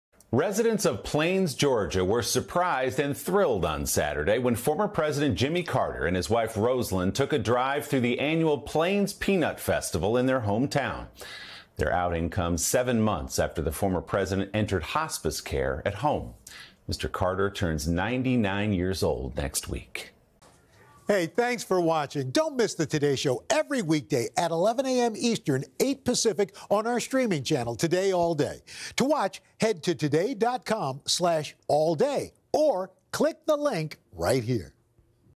Jimmy, Rosalynn Carter make rare public appearance in Georgia
Former President Jimmy Carter and his wife Rosalynn surprised and thrilled residents of Plains, Georgia when they took a drive through the annual Plains Peanut Festival. The Carter’s outing comes seven months after the former president entered hospice care.